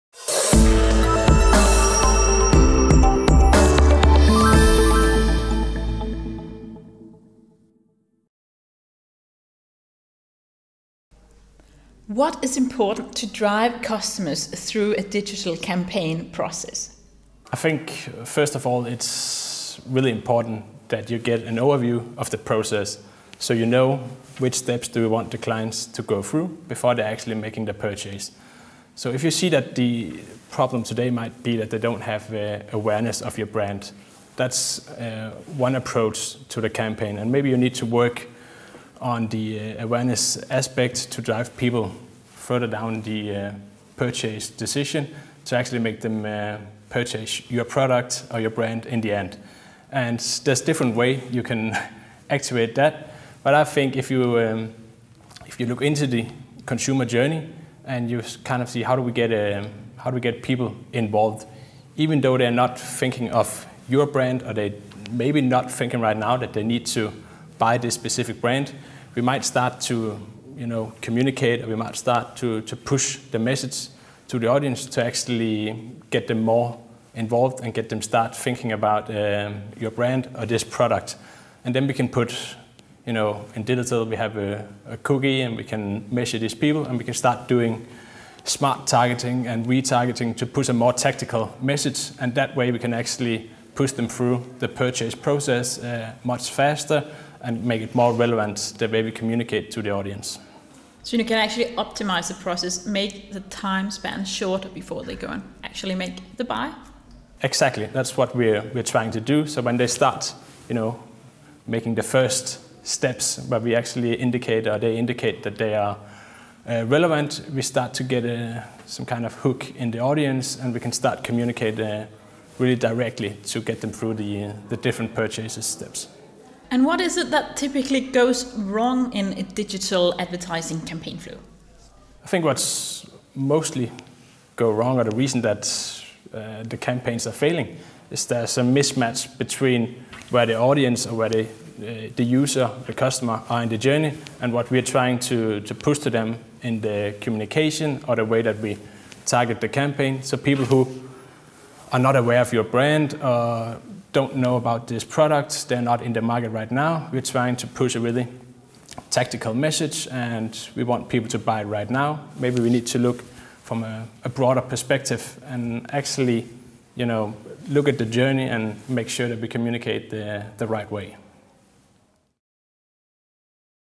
How to improve customer loyalty and get better financial results (2 of 5)
Ulrik Kristensen, Digital Director, MediaCom gives advice on how to use the new and increasing digital opportunities to engage in a dialogue with your consumer all the way through the customer journey.

Learn this:
- How to spot opportunities within digital advertising
- How to drive Customers through a digital campaign process
- What Digital advertising can do for your business
- How to exploit Social Media for advertising purpose
- How to get a message across to the audience

"Everything is moving into digital now, and this means vast possibilities for the digital advertizer," says Ulrik Kristensen, digital director, MediaCom and digital marketing expert. "Use the opportunities to follow your customer and communicate with your customer all the way through the consumer journey," is his advice.